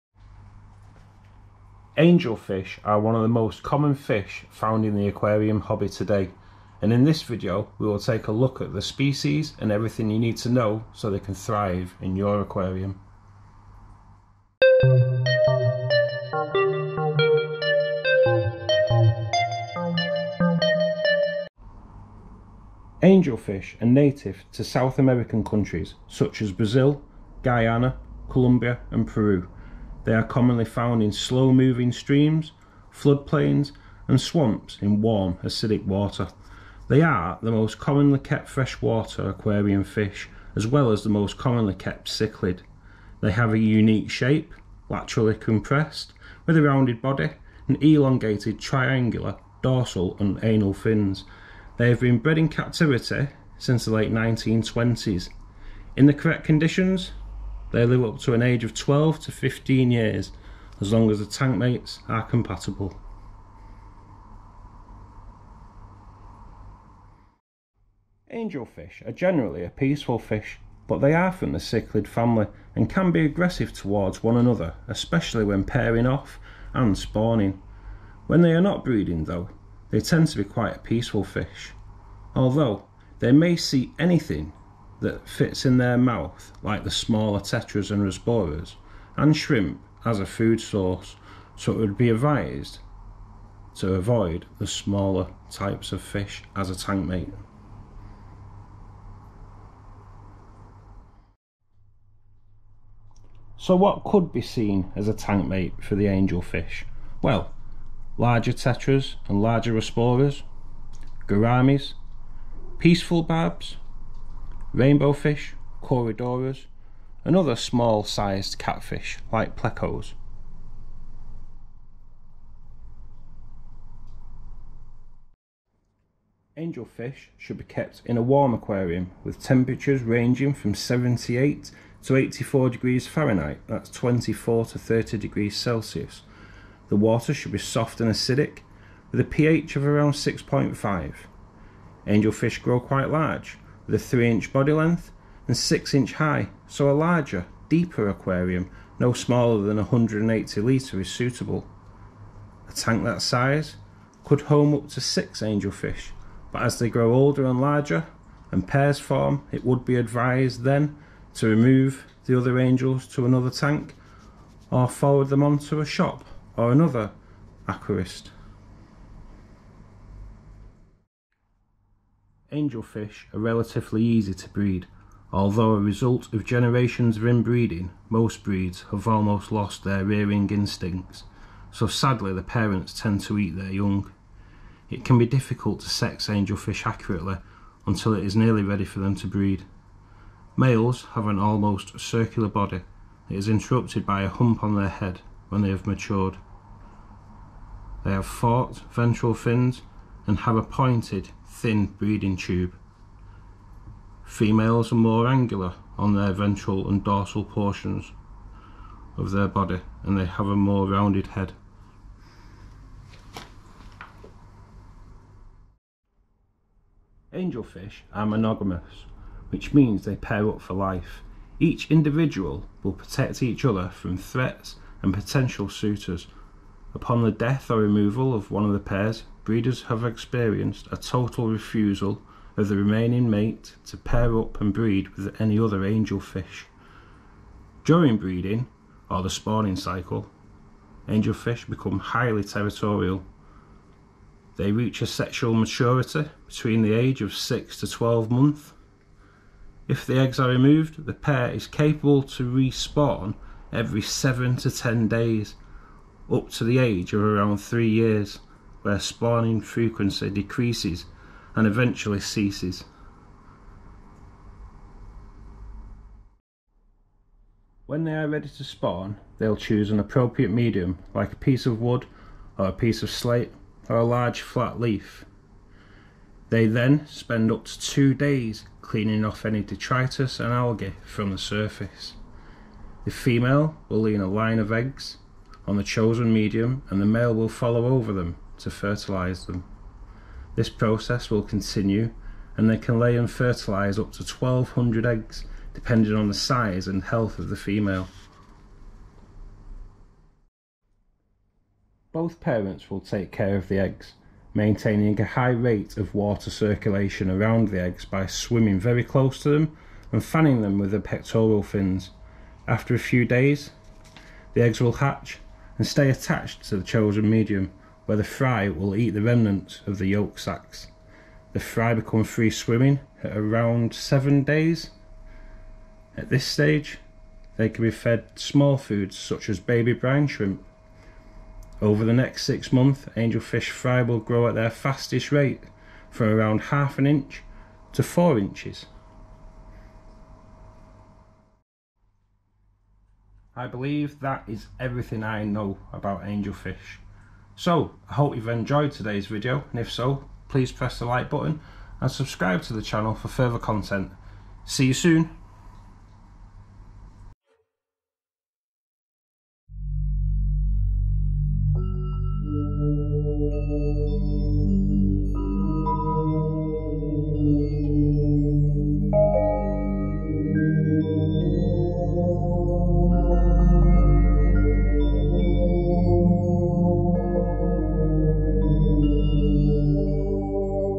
Angelfish Species Profile. Everything You Need To Know | BGN Aquariums
Are you looking for a showstopper in your aquarium? Look no further than angelfish! Known for their vibrant colors and flowing fins, angelfish make an impressive addition to any aquarium. Here is everything you need to know about angelfish:

Care: Angelfish are relatively low-maintenance and can live in a variety of water conditions. They need a peaceful environment and plenty of hiding spaces to feel comfortable. They should also have plenty of vegetation to graze on.

Breeding: Breeding angelfish can be challenging, but with the right environment and conditions, it is possible. Provide plenty of vegetation and hiding spaces, and maintain ideal water conditions.

Tank Mates: Angelfish can be territorial and aggressive when breeding, so it’s best to keep them in a tank by themselves or with other peaceful fish.

Origins: Angelfish are native to South America and can be found in many tropical rivers and streams. They have adapted to a variety of water conditions and can be found in a range of colors.

Now that you know more about angelfish, get ready to enjoy their beauty and grace in your aquarium!

ALWAYS REMEMBER: Find out as much information on any animal, and make sure you can offer then ideal living conditions before you take their lives in your hands.